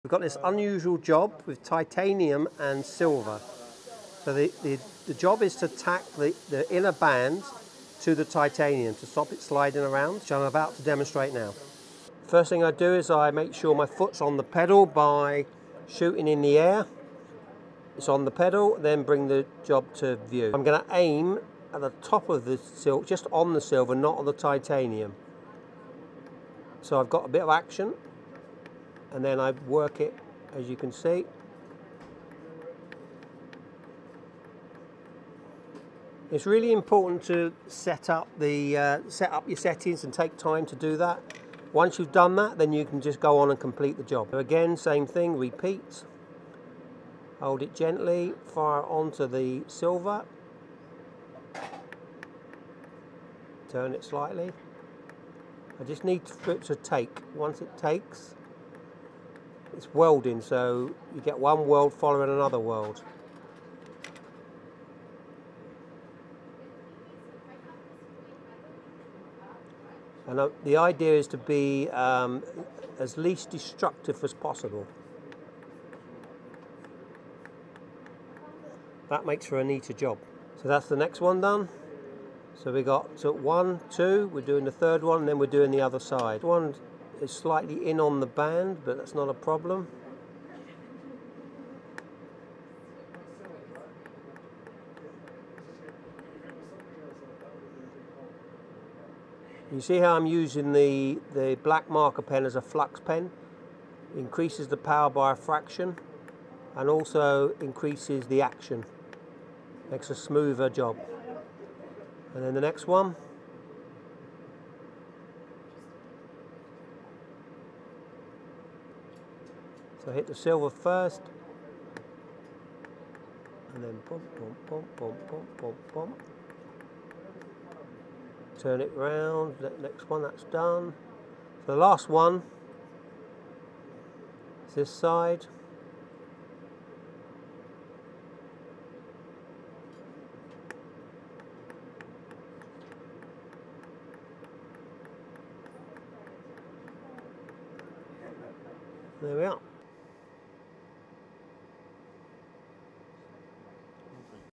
Laser welding together a silver and titanium to make a ring
As part of our travelling laser welding workshops, we spent Wednesday at BAJ running three hands-on sessions with their students and tutors. ⚡